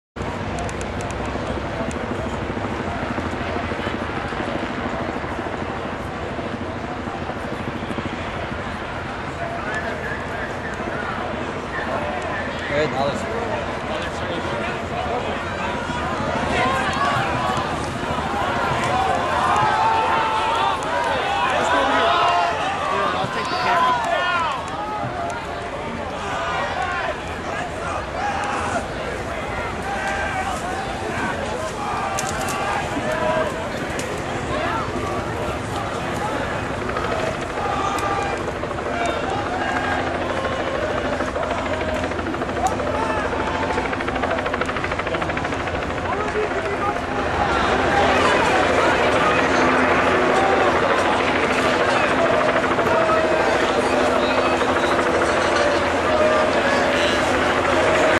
St. Paul AntiWar March 10
Blocked from crossing, protesters sit down again at the Cedar St. bridge.  Police move in to seize banners and signs and make arrests.